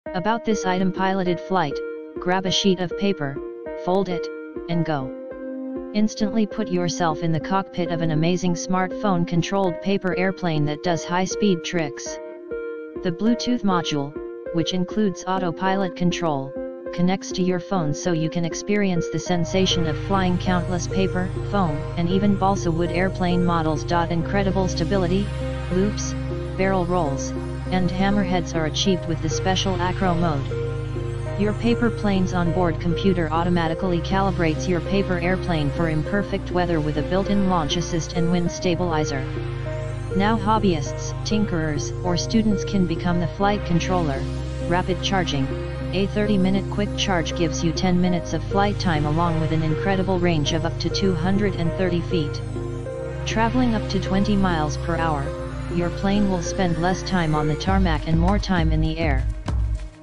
POWERUP 4.0 The Next-Generation Smartphone Controlled Paper Airplane Kit, RC Controlled. Easy to Fl
POWERUP 4.0 The Next-Generation Smartphone Controlled Paper Airplane Kit, RC Controlled. Easy to Fly with Autopilot & Gyro Stabilizer. For Hobbyists, Pilots, Tinkerers. STEM Ready with DIY Modular Kit

PILOTED FLIGHT - Grab a sheet of paper, fold it, and go! Instantly put yourself in the cockpit of an amazing smartphone controlled paper airplane that does high speed tricks. The bluetooth module, which includes autopilot control, connects to your phone so you can experience the sensation of flying countless paper, foam, and even Balsa Wood airplane models.
INCREDIBLE STABILITY - Loops, Barrel Rolls, & Hammerheads are achieved with the special Acro Mode! Your paper plane’s onboard computer automatically calibrates your paper airplane for imperfect weather with a built-in launch assist and wind stabilizer. Now hobbyists, tinkerers, or students can become the flight controller!
RAPID CHARGING - A 30 minute quick charge gives you 10 minutes of flight time along with an incredible range of up to 230ft! Traveling up to 20mph, your plane will spend less time on the tarmac and more time in the air. Simply connect the included micro USB cable to a charging port and plot out your next journey.
STEM READY - Experience the aerodynamic forces that make flight possible through various wing configurations. Lift, drag, thrust, and gravity are learned first hand. Purchase the Night-Flight Kit and Accessory Kit to see how different parts create a new experience. Great for an outdoor class or exploration together with family.
DURABLE - A near indestructible heavy duty carbon fiber body includes a nylon reinforced nose and motor mount. The POWERUP 4.0 paper airplane set is designed to withstand bumps, bangs, and even an unexpected crash. If your paper plane flies out of range, the onboard computer will safely land itself! Includes a one year warranty.